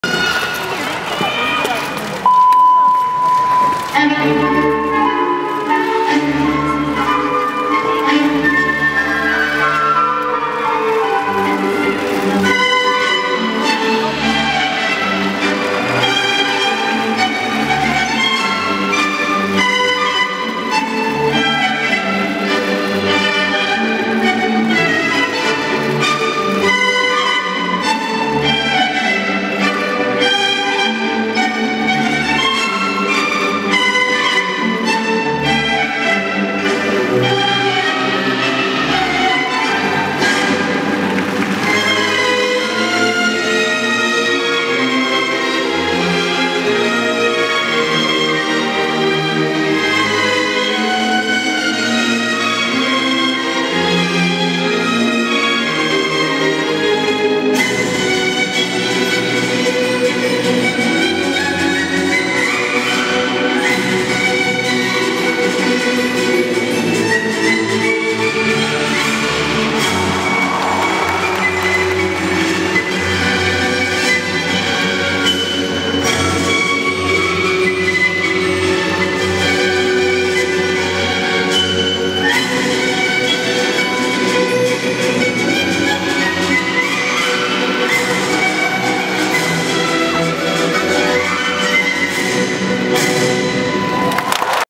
인천아시안게임 리듬체조 개인종합 예선 손연재 (Son Yeon Jae)후프
2014년 10월 1일
인천아시안게임 리듬체조 개인종합 예선 손연재 후프
17.850점